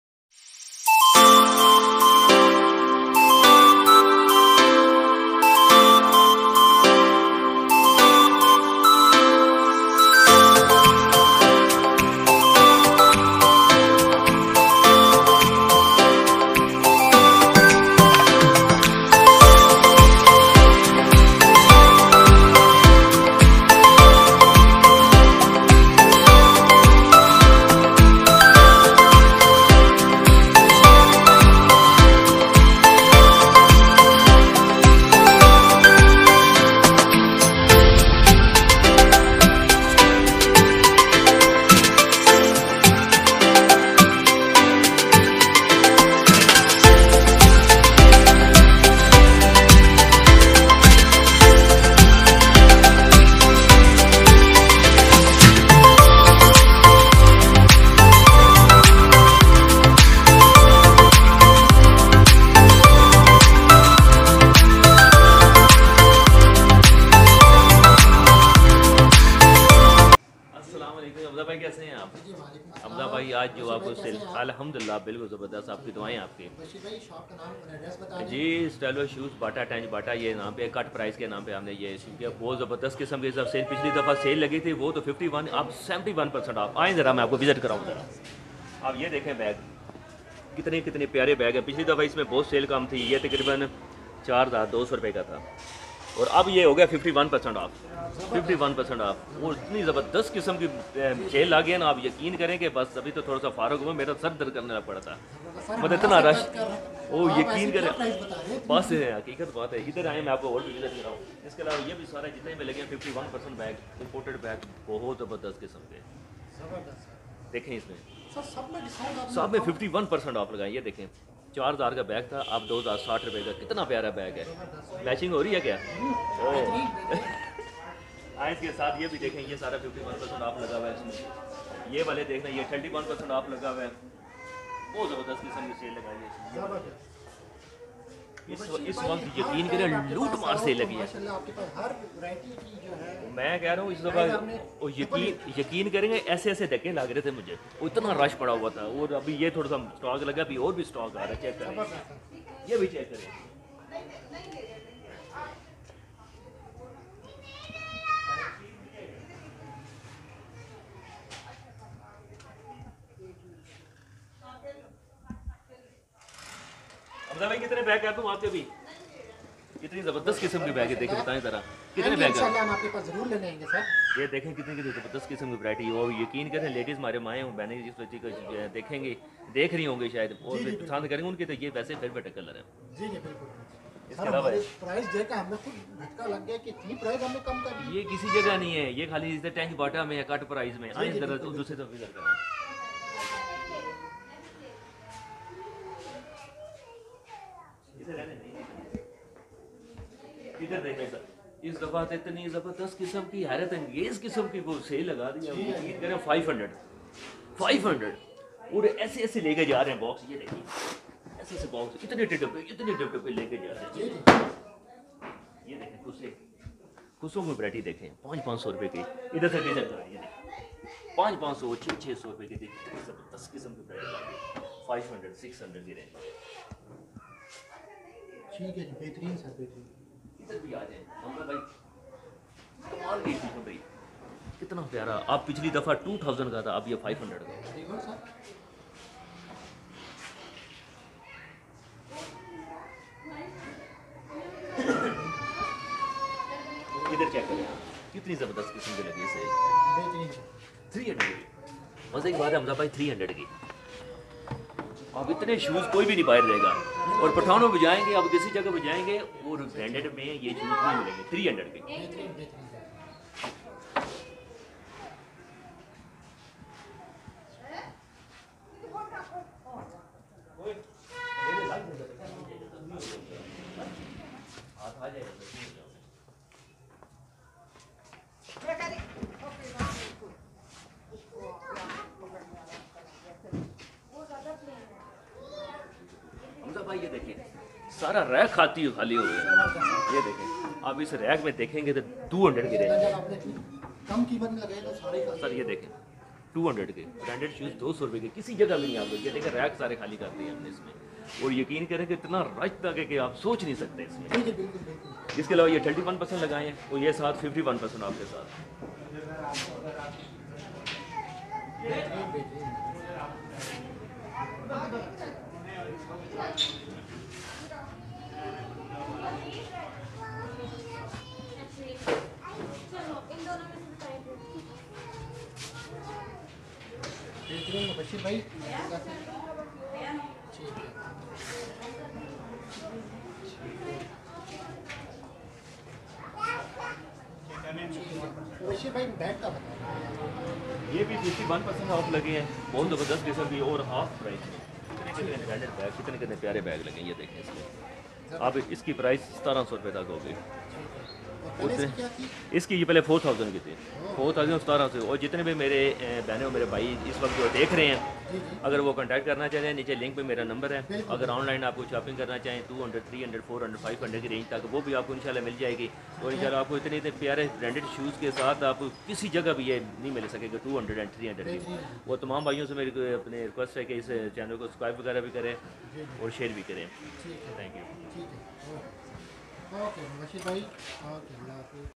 Stylo shoes sale 51% off 2022/winter shoes sale/sneaker,court shoes,heels,moccasin &flats#shopping H
Shopping name. stylo shoes 👞 👟
Address Tench Bhetta last stop Rawalpindi